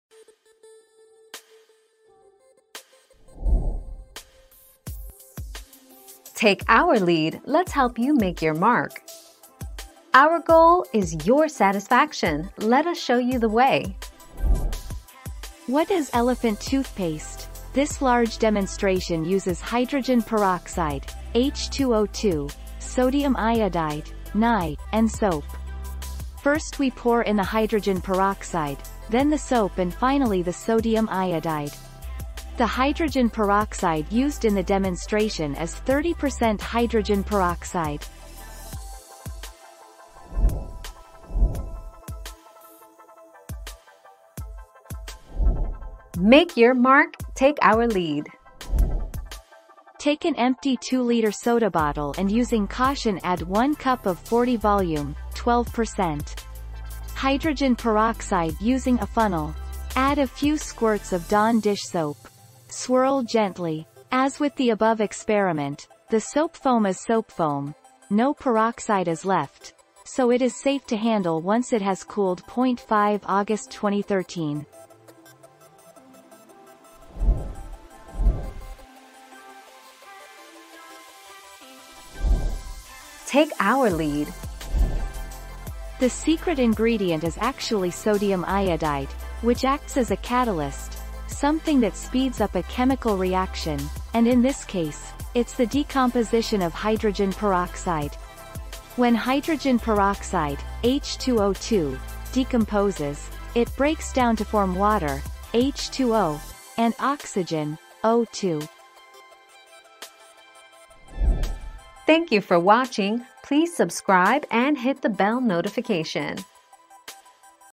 What chemicals do you use to make elephant toothpaste?
00:00 - What chemicals do you use to make elephant toothpaste?
00:42 - Can you use 12 hydrogen peroxide for elephant toothpaste?
01:18 - What is the best catalyst for elephant toothpaste?

Laura S. Harris (2021, March 23.) What chemicals do you use to make elephant toothpaste?
    AskAbout.video/articles/What-chemicals-do-you-use-to-make-elephant-toothpaste-233717